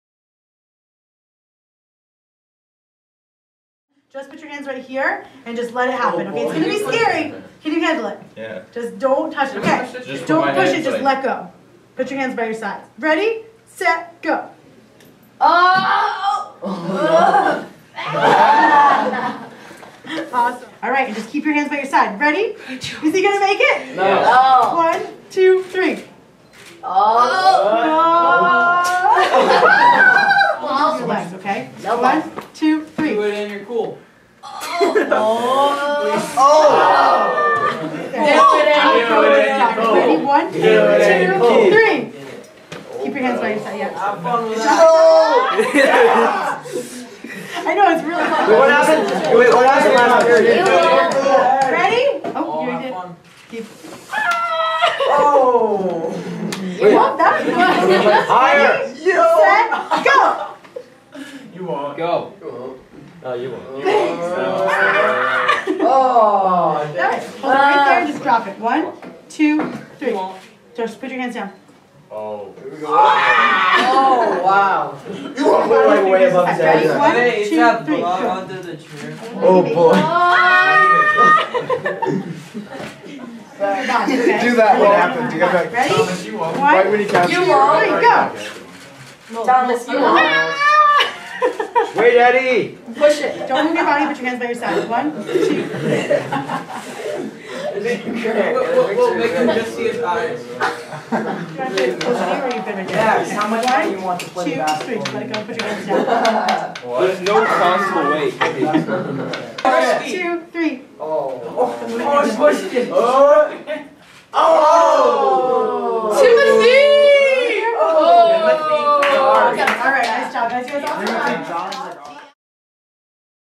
Demonstrating the Law of Conservation of Energy at Cardigan
Cardigan's Physical Science class demonstrates the law of conservation of energy, which states that energy cannot be created nor destroyed, only transformed from one form to another.